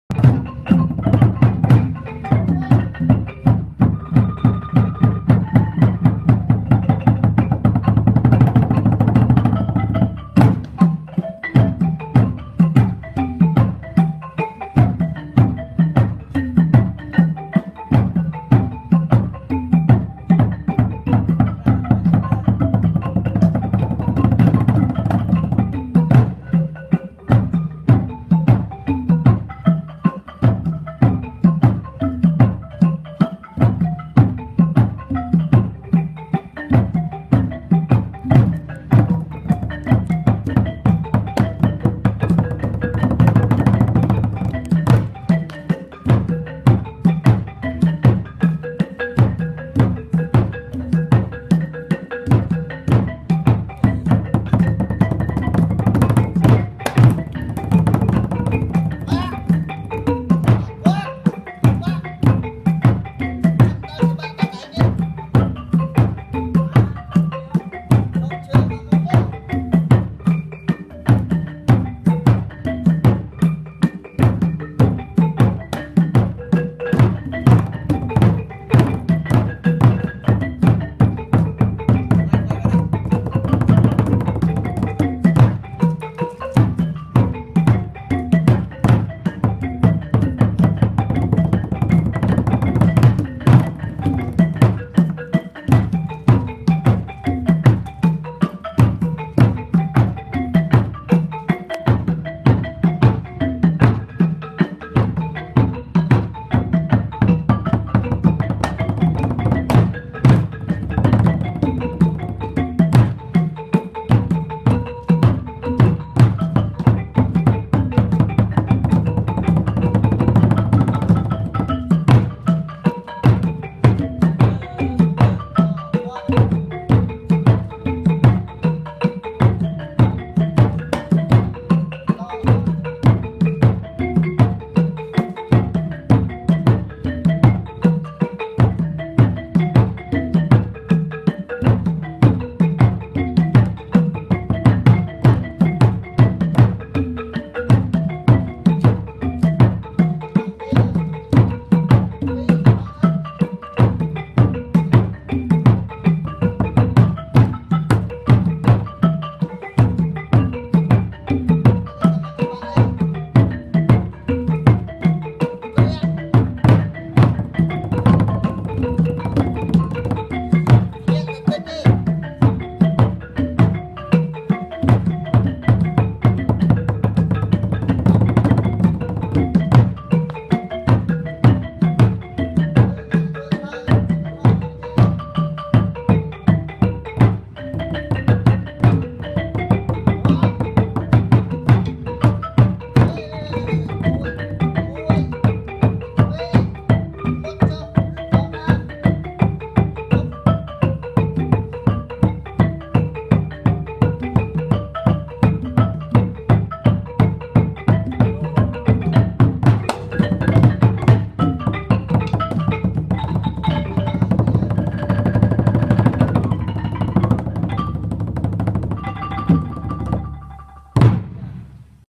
ក្បាច់បាតយក្សបែប វត្តស្វាយអណ្ដែត
នេះជាការហាត់សម រាំថ្វាយយក្សវត្តស្វាយ របស់ក្រុមសិល្បៈលោកគ្រូ ផល សារ៉ាវុធ។ ថតដោយ៖ ម៉ៅ សុខចំរើន